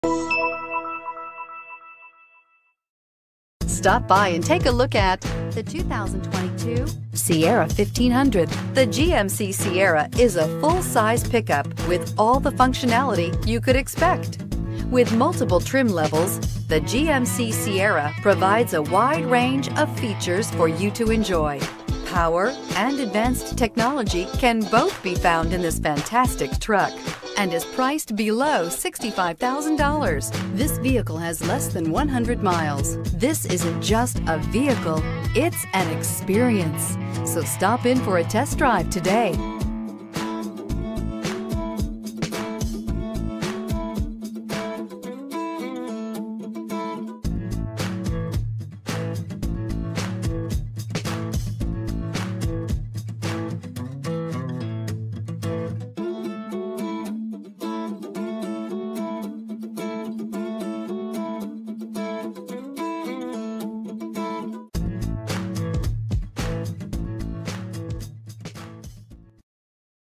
2022 GMC Sierra 1500 G563570
2022 GMC Sierra 1500 Elevation

This Quicksilver Metallic 2022 GMC Sierra 1500 Elevation might be just the crew cab for you.  It comes with a 8 Cylinder engine.  This one's on the market for $60,650.  For a good-looking vehicle from the inside out, this crew cab features a sharp quicksilver metallic exterior along with a black interior.  This is the vehicle you have been waiting for!